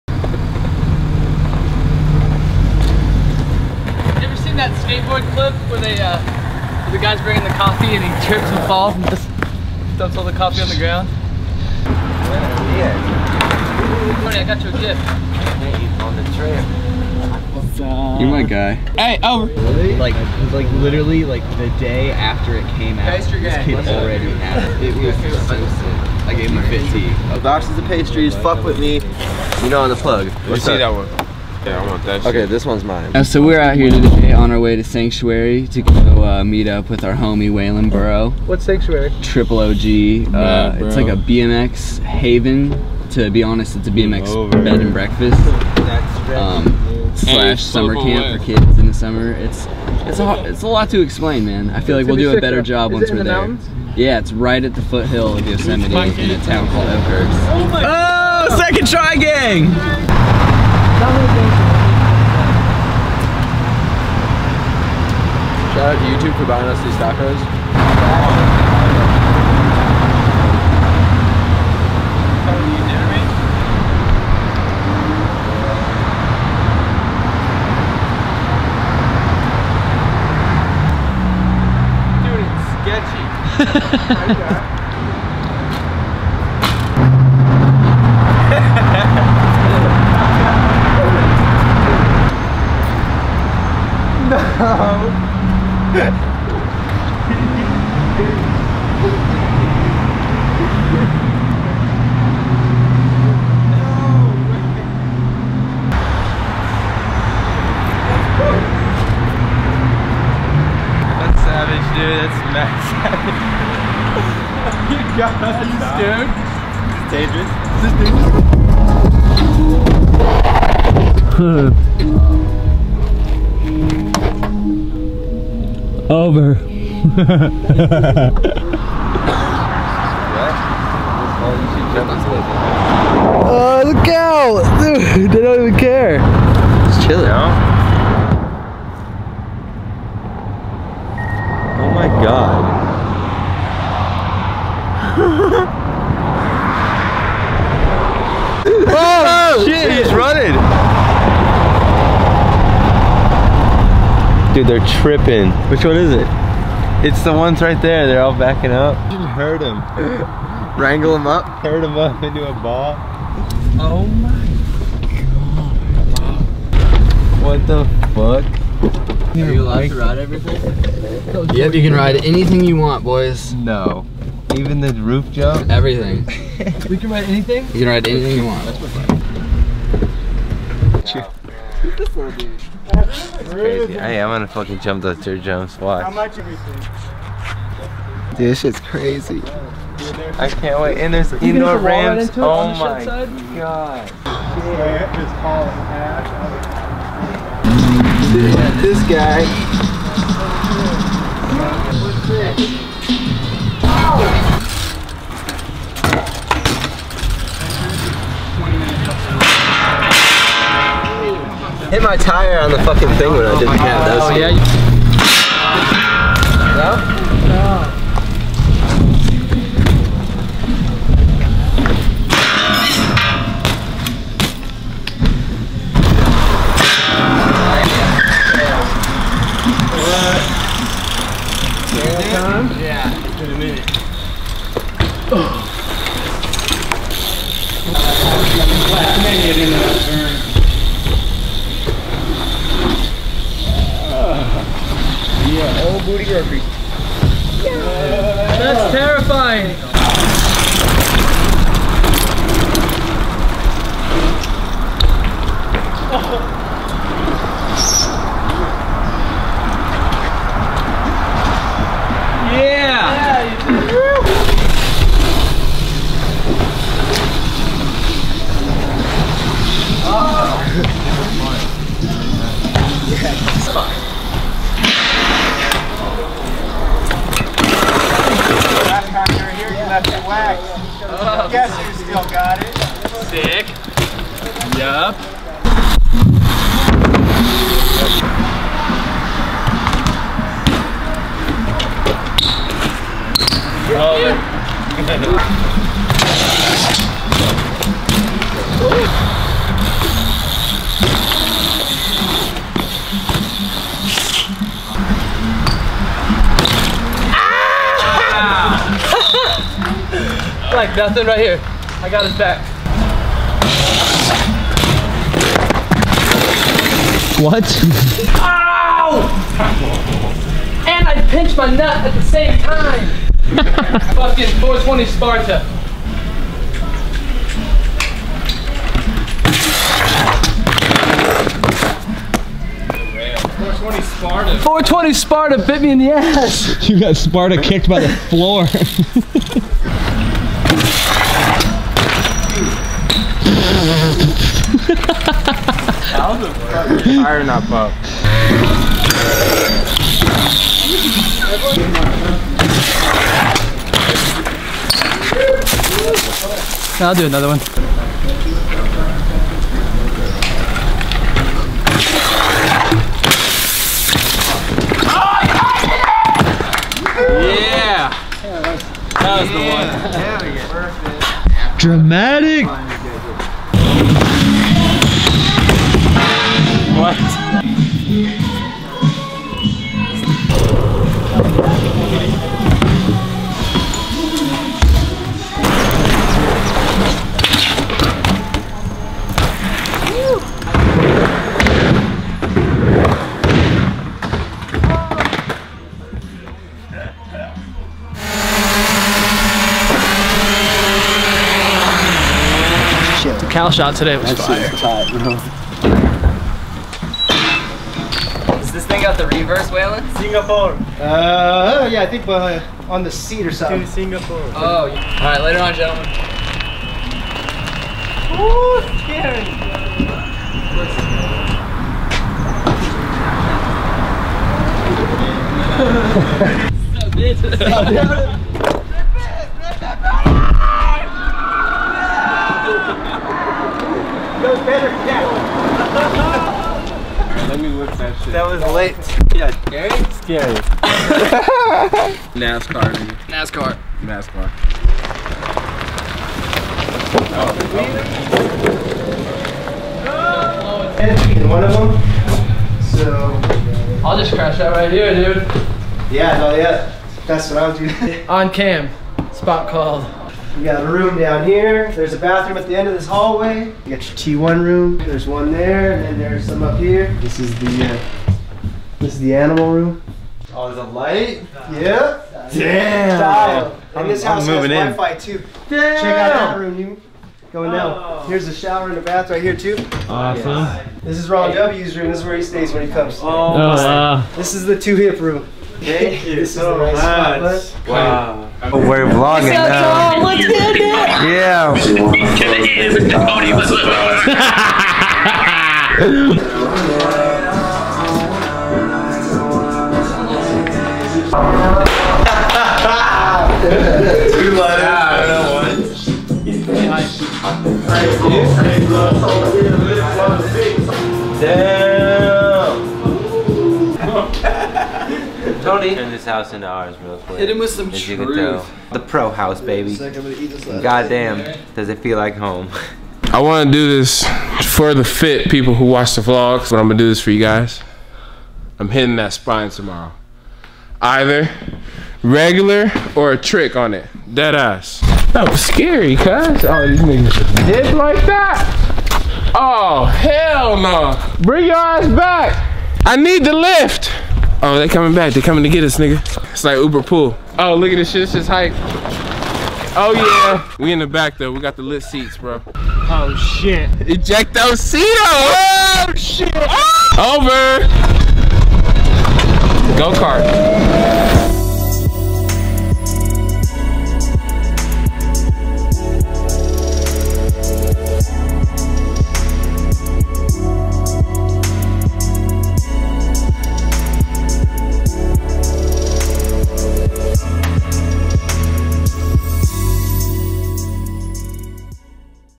Fitbikeco. F-LOG 23 - The Sanctuary
The gang visits the Sanctuary for a little BMX get away.

Featuring:
Austin Augie - @austinaugie
Brandon Begin - @brandonbegin
Charles Littlejohn - @charles_littlejohn
Brandon James - @cheifbrandoooo
Ethan Corriere - @ethancorriere
Stephan August - @bootygroceries
Yumi Tsukuda - @yumingrow

Thanks @sanctuary_bmx